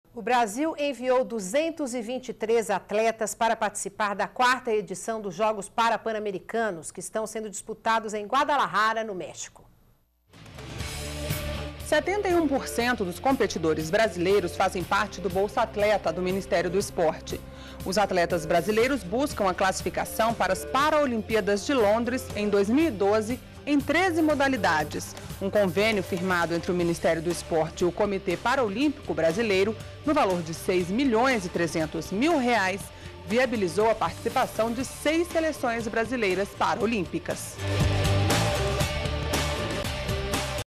Brasil é representado por 223 atletas nos Jogos Parapan-Americanos no México
NBR Notícias - 16.11.11: O Brasil enviou 223 atletas para participar da 4° edição dos Jogos Parapan-Americanos, que estão sendo disputados em Guadalajara, no México.